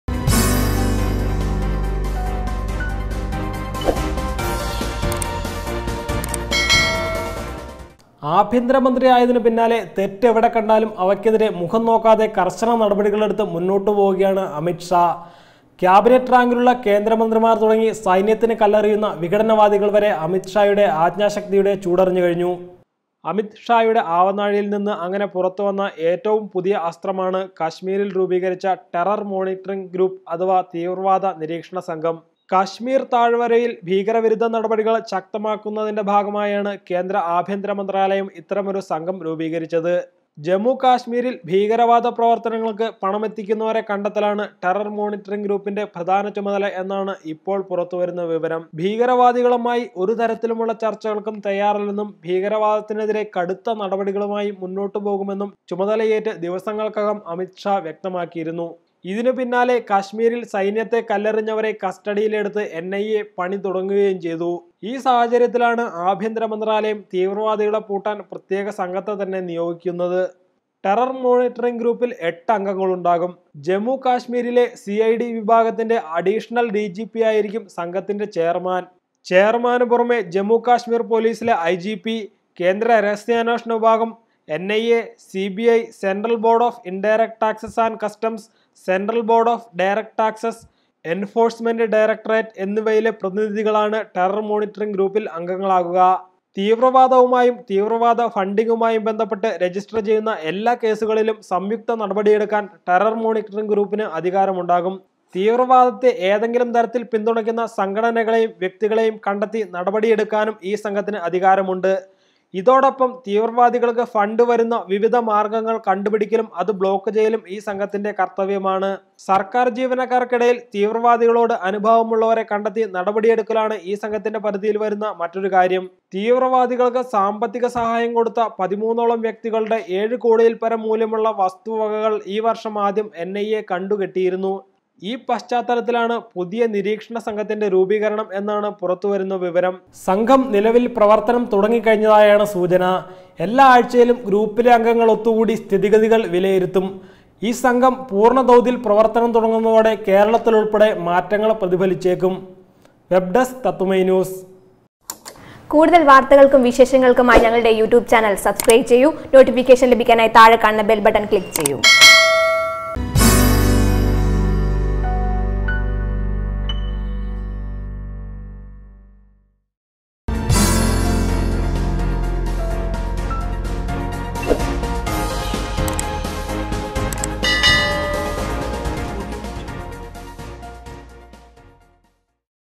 തീവ്രവാദികളെ ഒതുക്കാന്‍ ടെറര്‍ മോണിറ്ററിംഗ് ഗ്രൂപ്പുമായി കേന്ദ്ര ആഭ്യന്തര മന്ത്രാലയം
കാശ്മീര്‍ താഴ്‌വരയില്‍ ഭീകര വിരുദ്ധ നടപടികള്‍ ശക്തമാക്കുന്നതിന്റെ ഭാഗമായാണ് കേന്ദ്ര ആഭ്യന്തരമന്ത്രാലയം ടെറര്‍ മോണിറ്ററിംഗ് ഗ്രൂപ്പ്  രൂപീകരിച്ചത്. ജമ്മുകാശ്മീരില്‍ ഭീകരവാദ പ്രവര്‍ത്തനങ്ങള്‍ക്ക് പണമെത്തിക്കുന്നവരെ കണ്ടെത്തലാണ് ടെറര്‍ മോണിറ്ററിംഗ് ഗ്രൂപ്പിന്റെ പ്രധാന ചുമതല ..